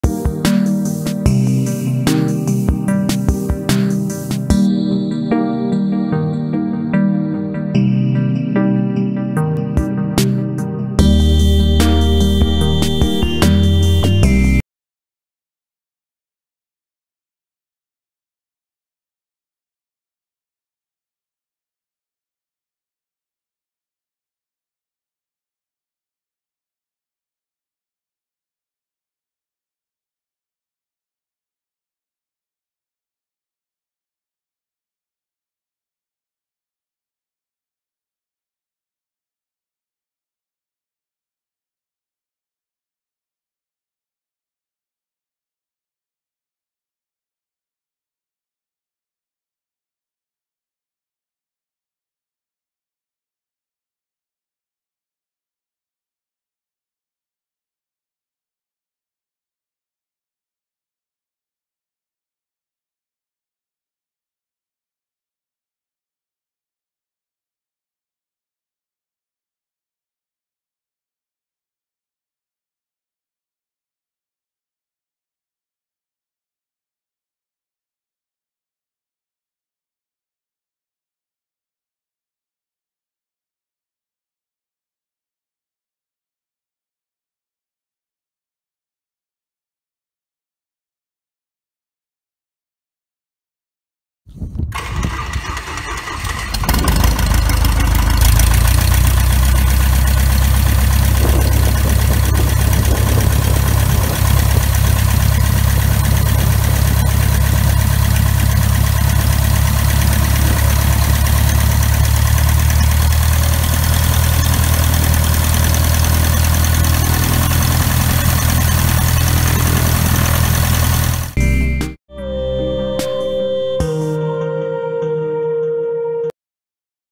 Cheetah Firewall Forward Restoration Part 2 - Grumman Style
Cheetah Firewall Forward Restoration Part 2 - Grumman Style:  User supplied photos makes this Part 2 of the Part 1 released in January of this year.  Video 838 in the Hit Parade.